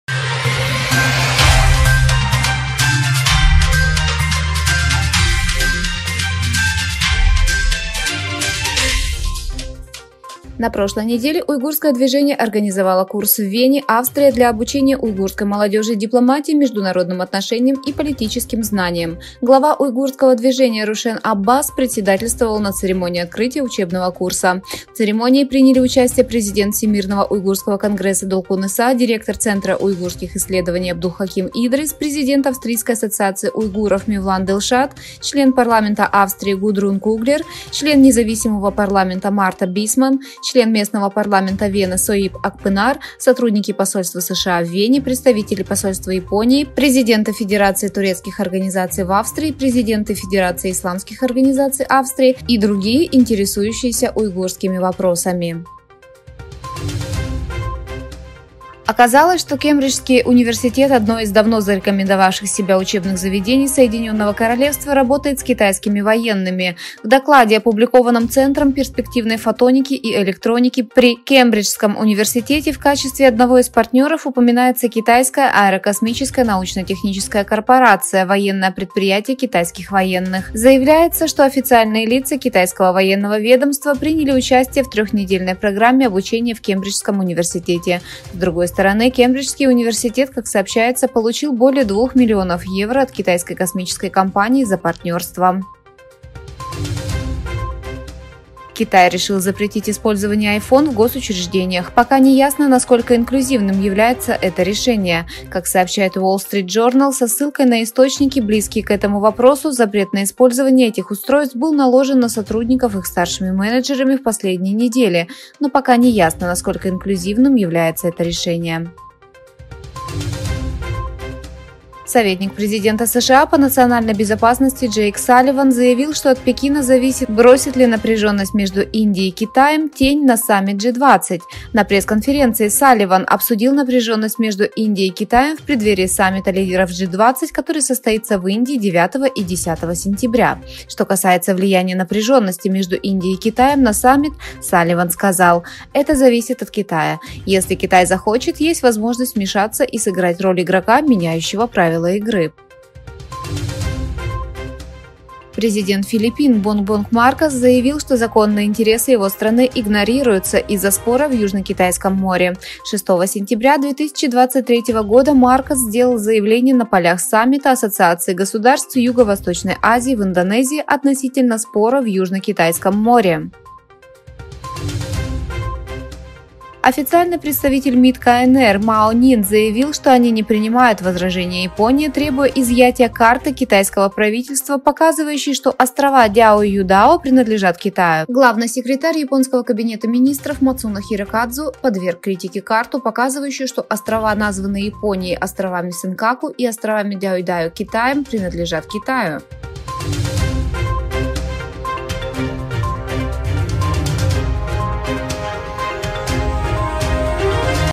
7/9/2023 Китай не принимает возражения Японии против карты
В Вене для уйгурской молодежи провели тренинг по дипломатии
На прошлой неделе Уйгурское движение организовало курс в Вене, Австрия, для обучения уйгурской молодежи дипломатии, международным отношениям и политическим знаниям. Глава Уйгурского движения Рушен Аббас председательствовал на церемонии открытия учебного курса. В церемонии приняли участие президент Всемирного уйгурского конгресса Долкун Иса, директор Центра уйгурских исследований Абдулхаким Идрис, президент Австрийской ассоциации уйгуров Мевлан Дилшат, член парламента Австрии Гудрун Куглер, член независимого парламента Марта Бисман, член местного парламента Вены Сойип Акпынар, сотрудники посольства США в Вене, представители посольства Японии, президенты Федерации турецких организаций в Австрии, президенты Федерации исламских организаций Австрии и другие интересующиеся уйгурскими вопросами.
Разоблачены отношения Кембриджского университета с китайскими военными
Оказалось, что Кембриджский университет, одно из давно зарекомендовавших себя учебных заведений Соединенного Королевства, работает с китайскими военными. В докладе, опубликованном Центром перспективной фотоники и электроники при Кембриджском университете, в качестве одного из партнеров упоминается Китайская аэрокосмическая научно-техническая корпорация (CASC), военное предприятие китайских военных. Заявляется, что официальные лица китайского военного ведомства приняли участие в трехнедельной программе обучения в Кембриджском университете. С другой стороны, Кембриджский университет, как сообщается, получил более 2 миллионов евро от китайской космической компании за партнерство.
Китай запретил использование айфонов в госучреждениях
Китай решил запретить использование iPhone в госучреждениях. Пока не ясно, насколько инклюзивным является это решение. Как сообщает Wall Street Journal со ссылкой на источники, близкие к этому вопросу, запрет на использование этих устройств был наложен на сотрудников их старшими менеджерами в последние недели, но пока не ясно, насколько инклюзивным является это решение.
США: Пекин не должен влиять на саммит G20 из-за отношений с Индией
Советник президента США по национальной безопасности Джейк Салливан заявил, что от Пекина зависит, бросит ли напряженность между Индией и Китаем тень на саммит G20. На пресс-конференции Салливан обсудил напряженность между Индией и Китаем в преддверии саммита лидеров G20, который состоится в Индии 9 и 10 сентября. Что касается влияния напряженности между Индией и Китаем на саммит, Салливан сказал: «Это зависит от Китая. Если Китай захочет, есть возможность вмешаться и сыграть роль игрока, меняющего правила игры».
Президент Филиппин: Китай игнорирует наши законные интересы.
Президент Филиппин Бонгбонг Маркос заявил, что законные интересы его страны игнорируются из-за спора в Южно-Китайском море. 6 сентября 2023 года Маркос сделал заявление на полях саммита Ассоциации государств Юго-Восточной Азии (АСЕАН) в Индонезии относительно спора в Южно-Китайском море.
Китай не принимает возражения Японии против карты
Официальный представитель МИД КНР Мао Нин заявил, что они не принимают возражения Японии, требуя изъятия карты китайского правительства, показывающей, что острова Дяоюйдао принадлежат Китаю. Главный секретарь японского кабинета министров Мацуно Хирокадзу подверг критике карту, показывающую, что острова, названные Японией островами Сенкаку и островами Дяоюйдао Китаем, принадлежат Китаю